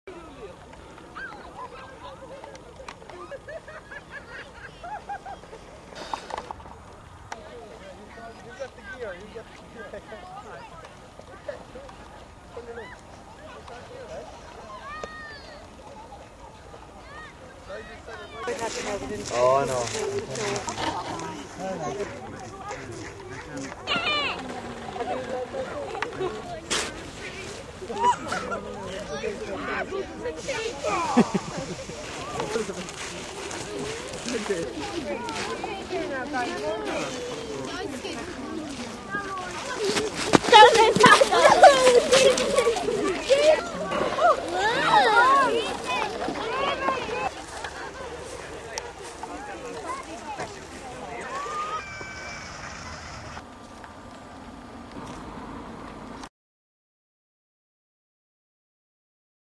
Lough Gur Ice Skating
The people in this video have immense local knowledge of Lough Gur and the lake.
Ice Skating in Lough Gur 2010,
The lake is frozen over which has led to local people Ice Skating.